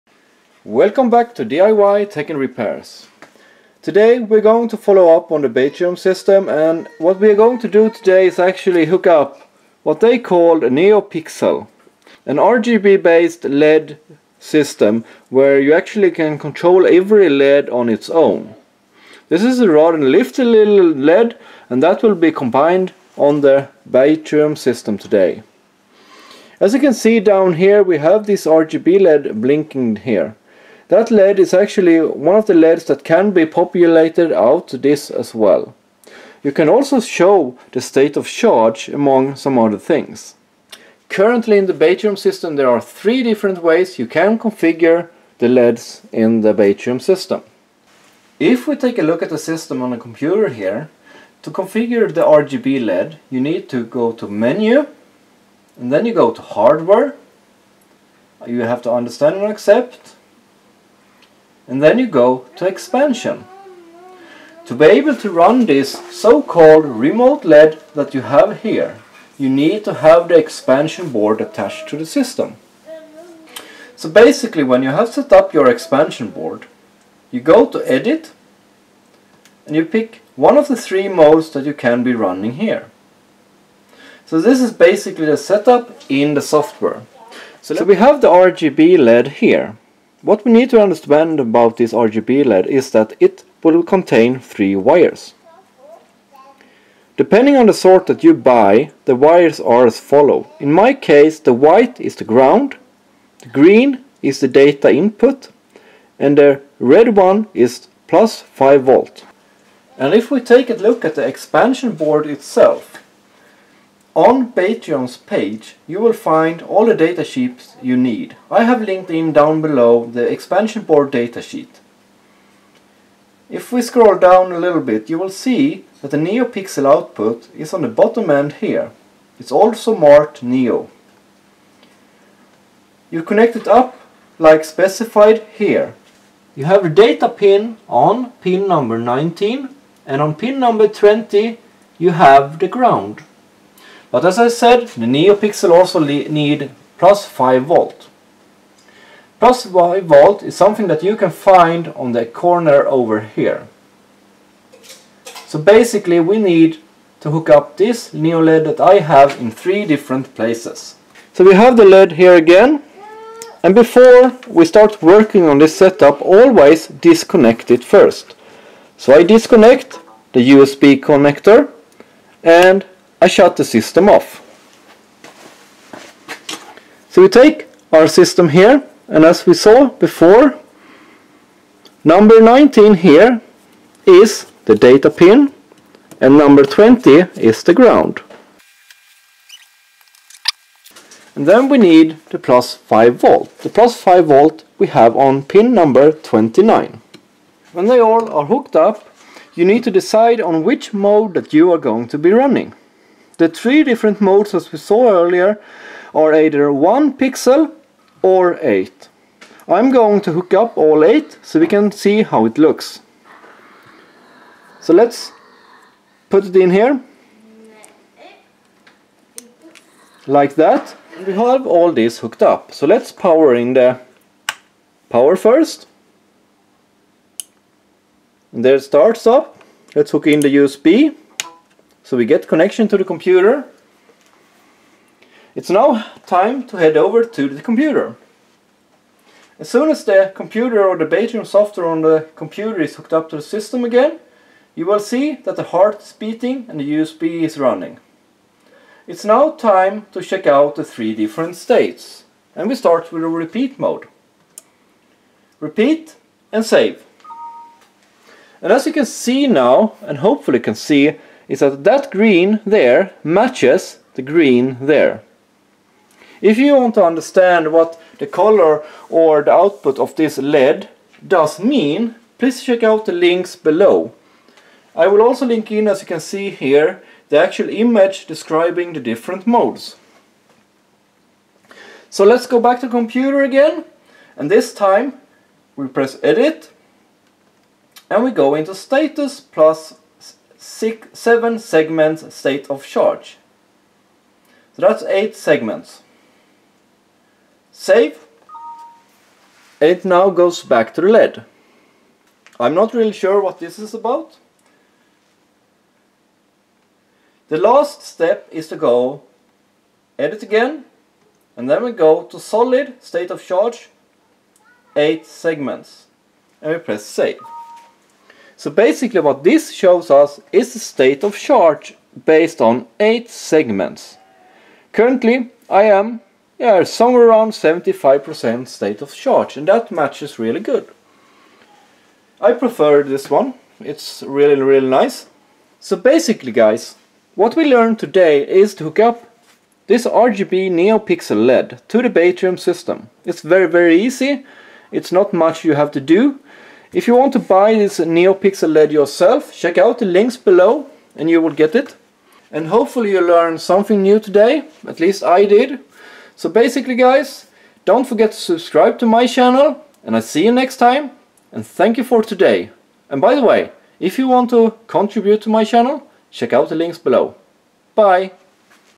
Batrium guide - Instal RGB LED
Batrium guide - Instal RGB LED that shows status and/or state of charge

SUPER DEAL on 18650 CELLS!

Tesla packs super deal!

By buying the stuff i link to will also yield some extra for me.